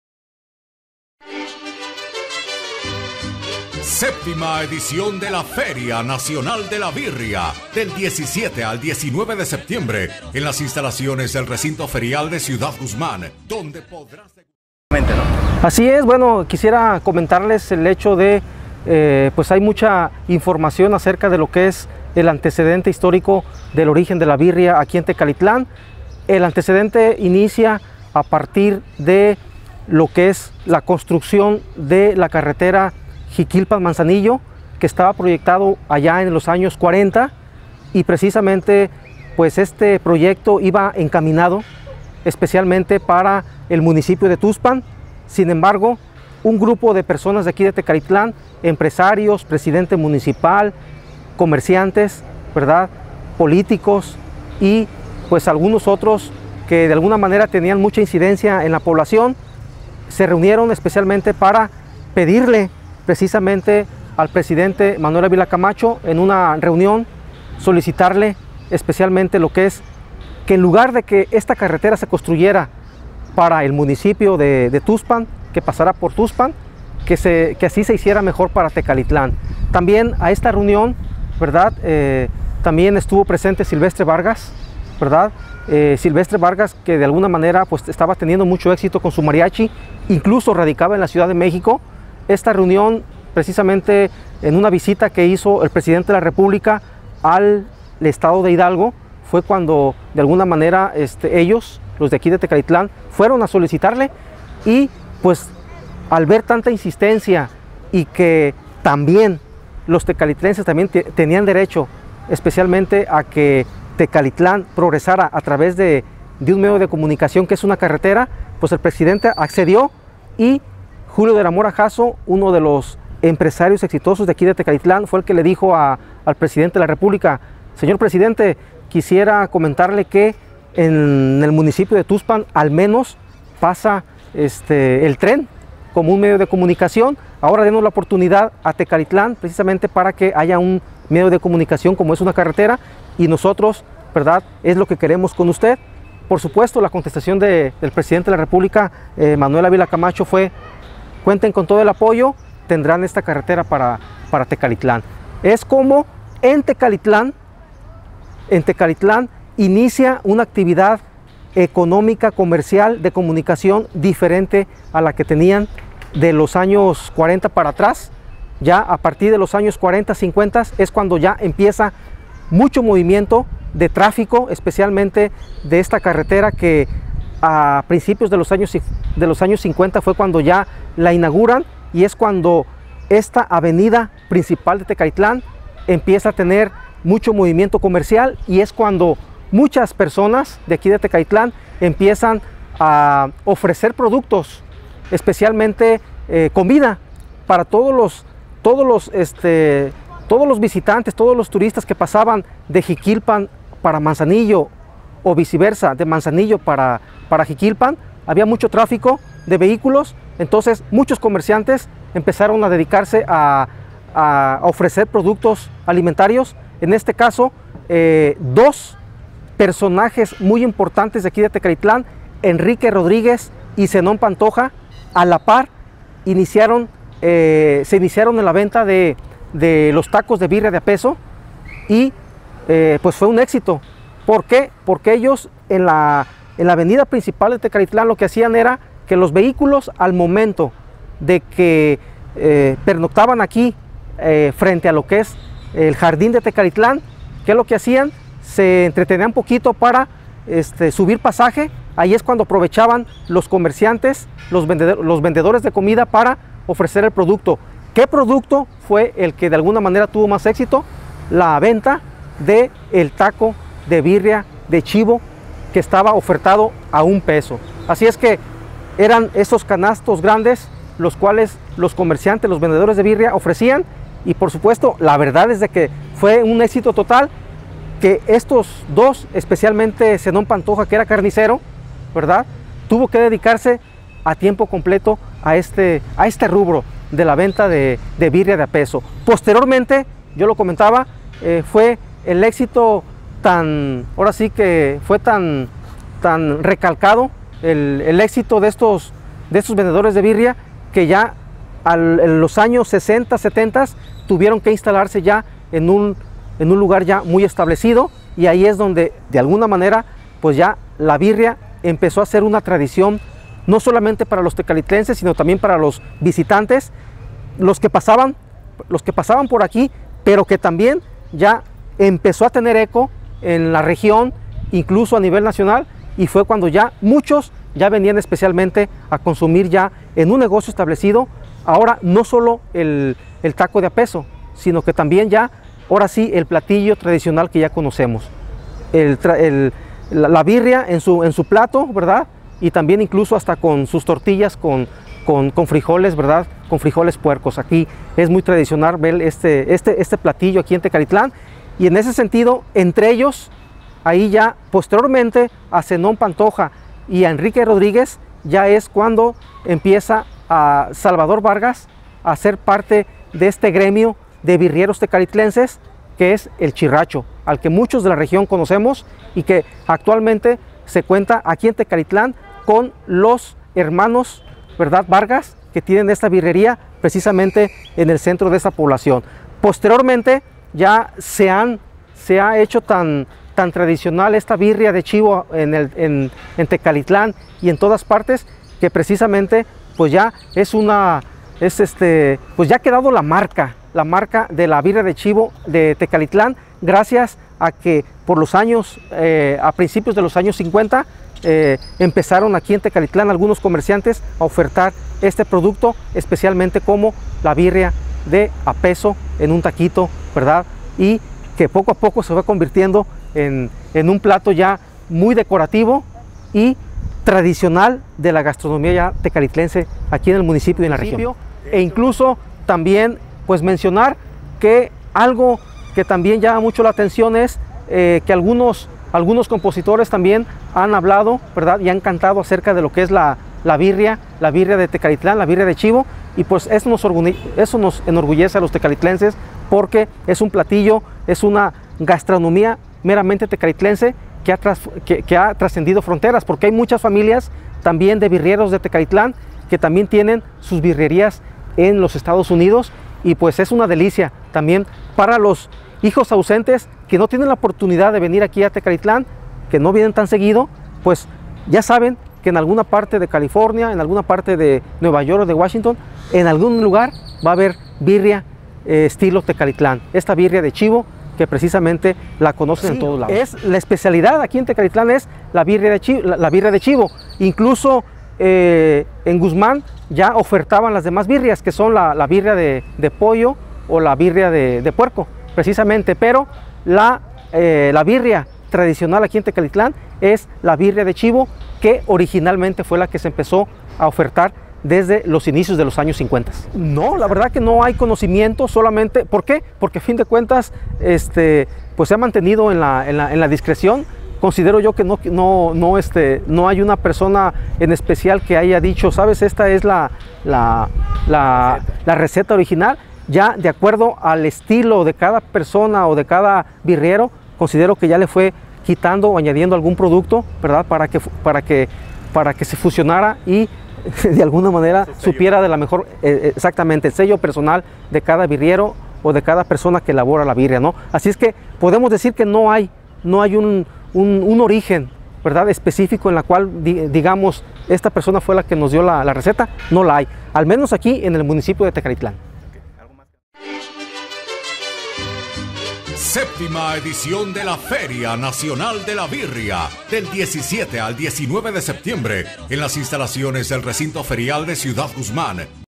Origen de la birria en Tecalitlán
Por la séptima edición de la Feria Nacional de la Birria, conozcamos el origen de este suculento platillo en Tecalitlán, por le cronista René Chávez.